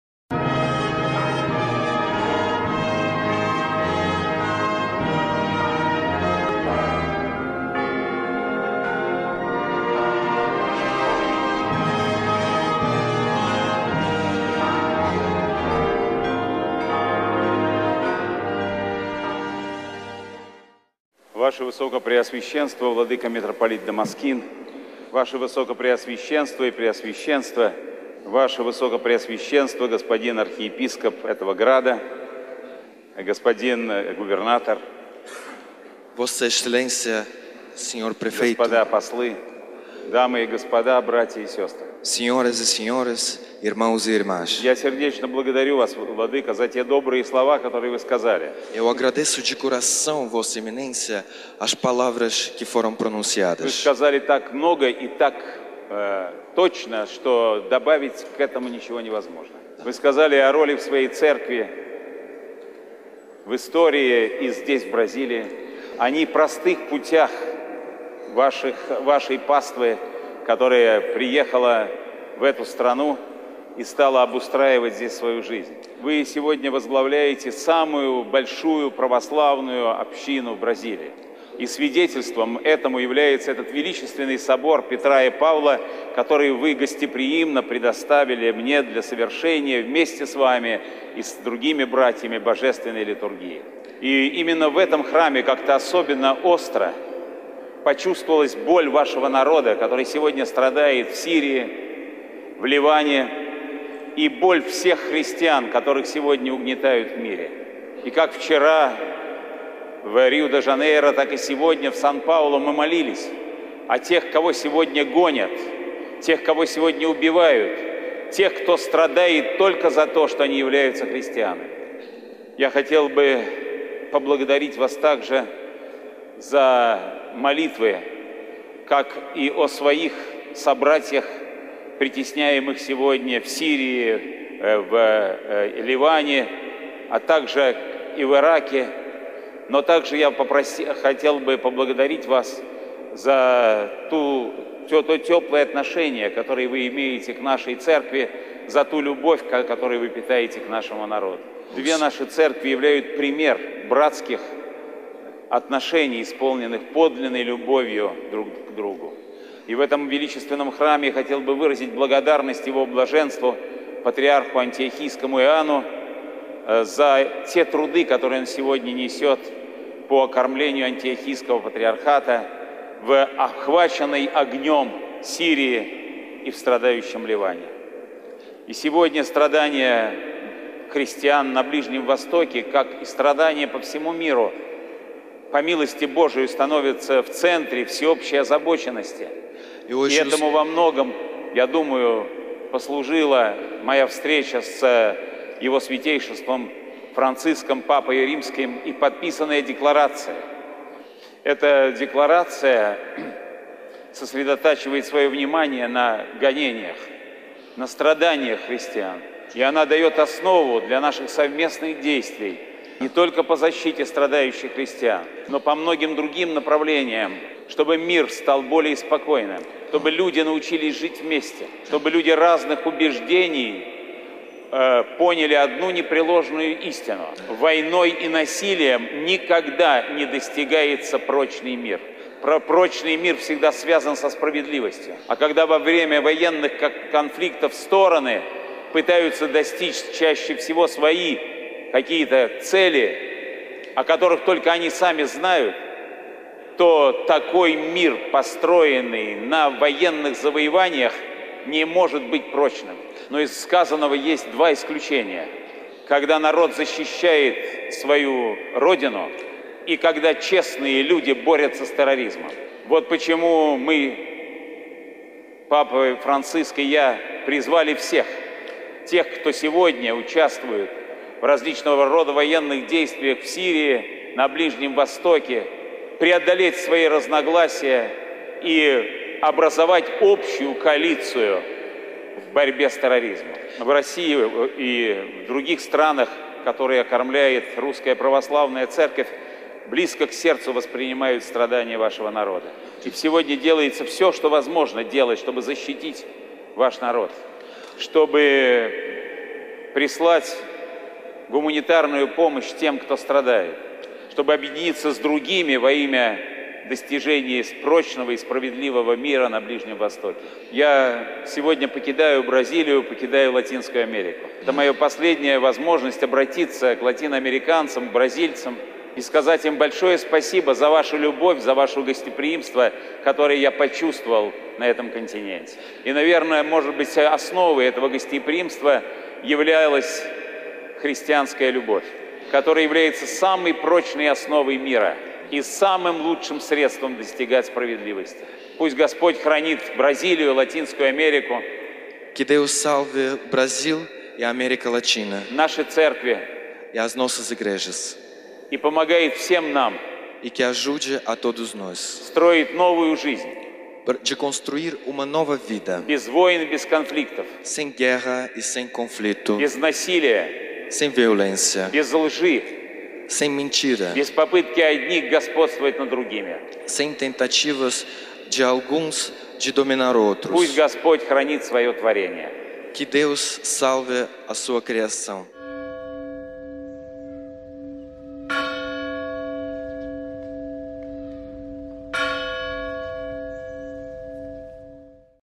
Слово в соборе святого апостола Павла в Сан-Паулу
Слово Святейшего Патриарха Московского и всея Руси Кирилла после Литургии в соборе святого апостола Павла в Сан-Паулу